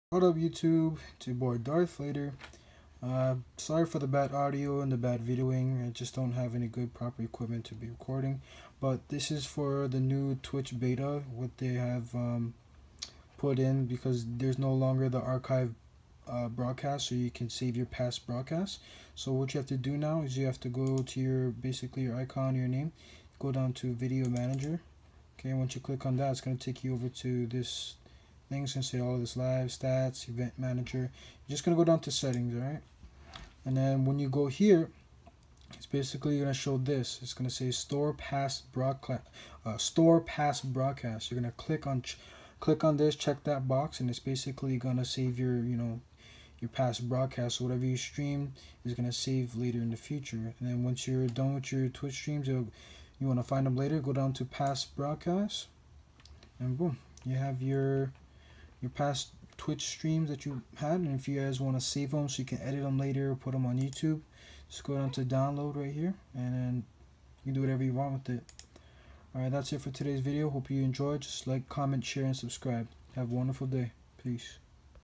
How to activate/save past broadcast on twitch!
HOW TO ACTIVATE OR SAVE PAST BROADCAST ON TWITCH FROM THE NEW BETA ON TWITCH!

 JUST GETTING STARTED ON MY YOUTUBE CHANNEL SO LET ME KNOW WHAT I SHOULD MAKE IN MY NEXT VIDEO AND WHAT I SHOULD IMPROVE ON?!